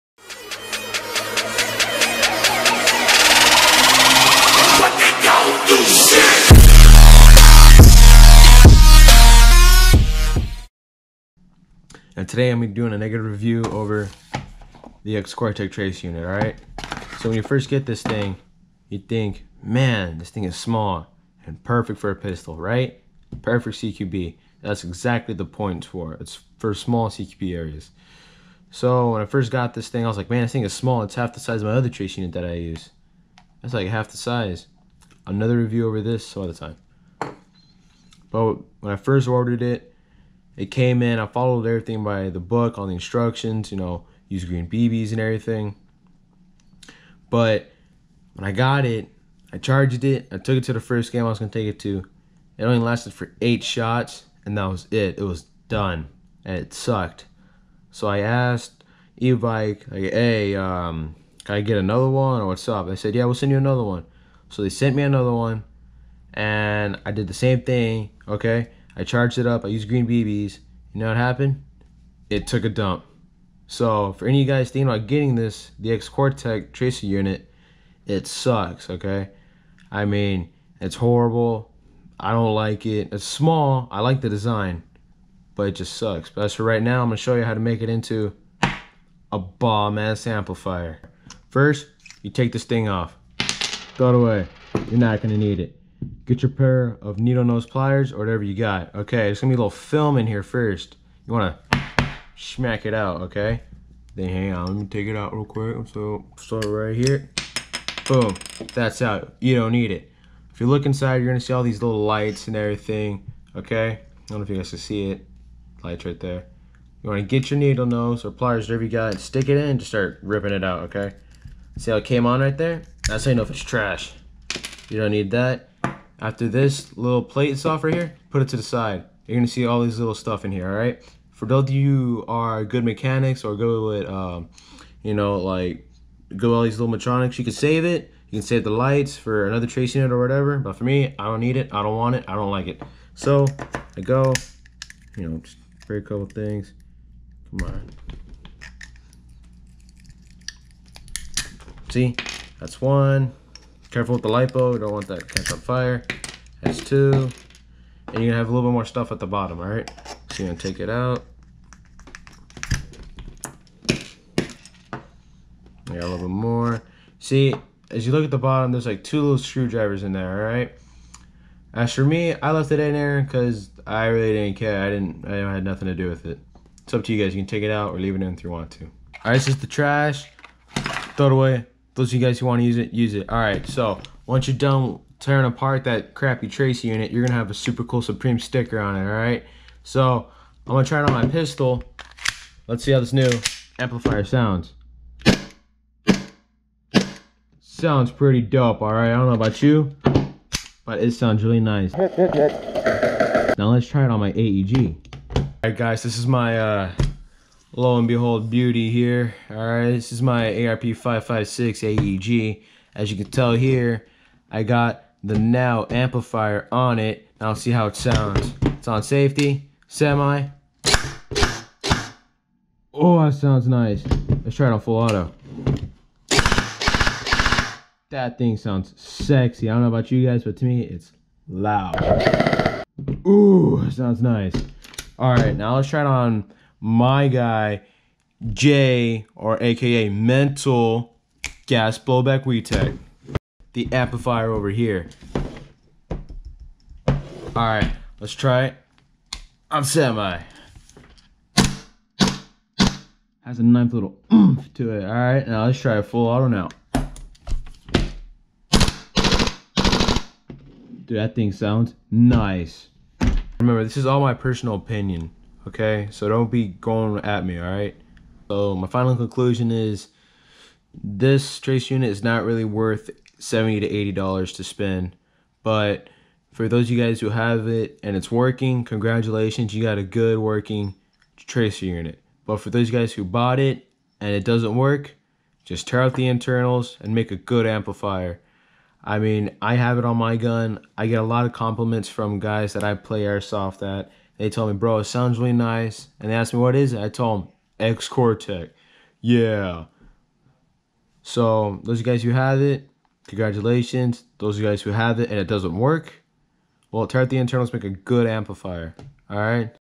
Xcore Tech 301 review / How to make a amplifier
THIS THING SUCK IT MAKES A BETTER AMPLIFIER THEN A TRACER UNIT.